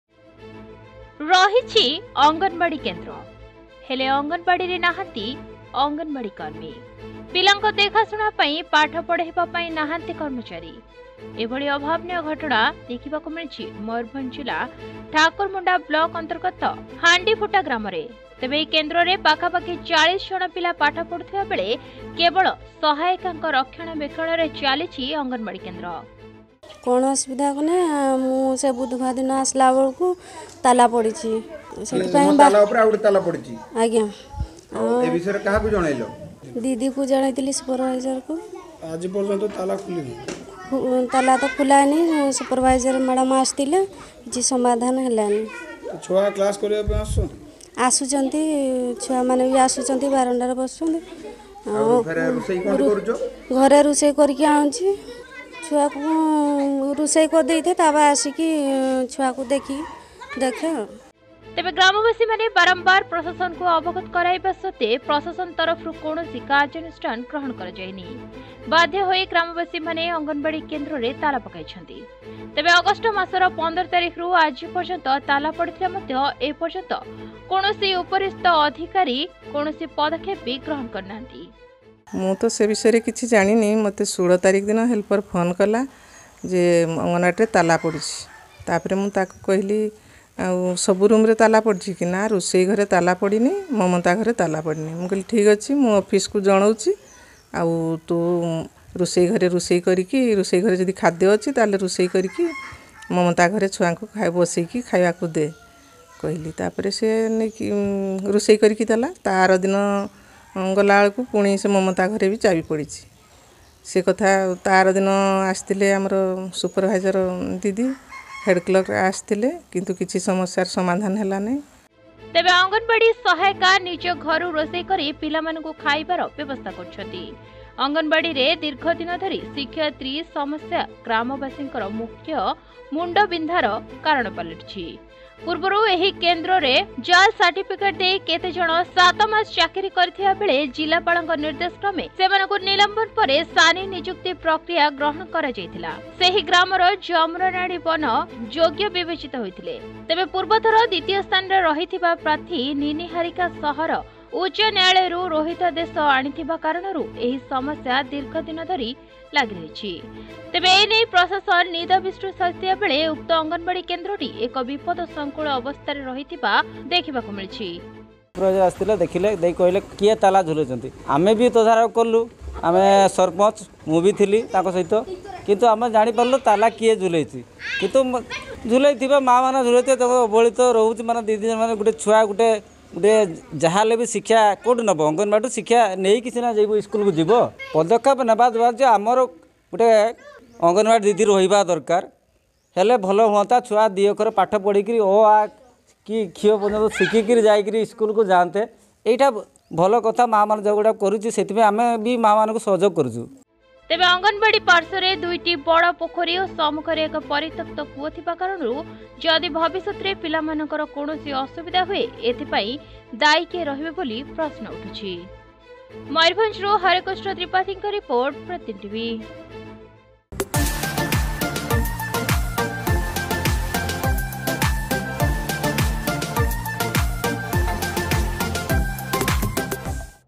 ୪ ବର୍ଷ ହେଲା ଅଙ୍ଗନବାଡ଼ି ଦିଦି ନାହାନ୍ତି ପିଲାମାନେ ଅଣ୍ଡା, ଭାତ ଖାଇ ପଳାଉଛନ୍ତି II PratidinTv
୪ ବର୍ଷ ହେଲା ଅଙ୍ଗନବାଡ଼ି ଦିଦି ନାହାନ୍ତି ପିଲାମାନେ ଅଣ୍ଡା, ଭାତ ଖାଇ ପଳାଉଛନ୍ତି I I